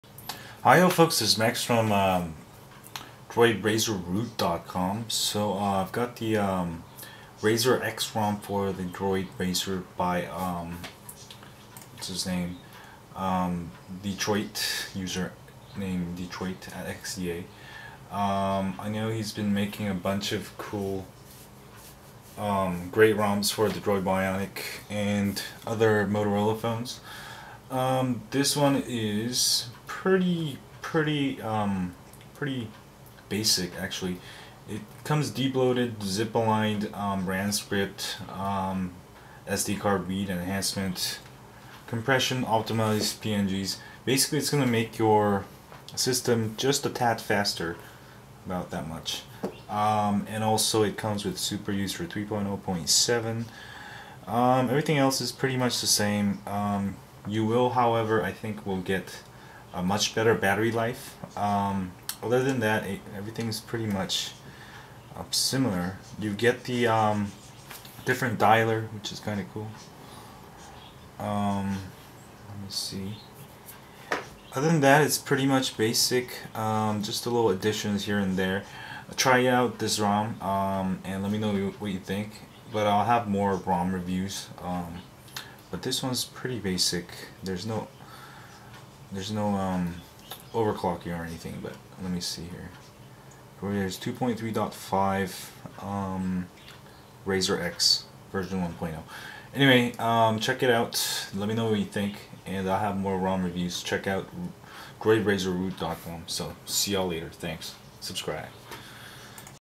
RAZRX ROM for Rooted Droid Razr!
Here's a quick overview of the RAZRX ROM for rooted Droid Razr!

Want to stay updated on Droid Razr rooting, ROMs, tips, and more?

Droid Razr Reference:

Note: For new ROMs, you will need a rooted Droid Razr and Safestrap/ClockworkMod Recovery installed!

PLEASE See Droid Razr root FAQ FIRST if you are new to all this: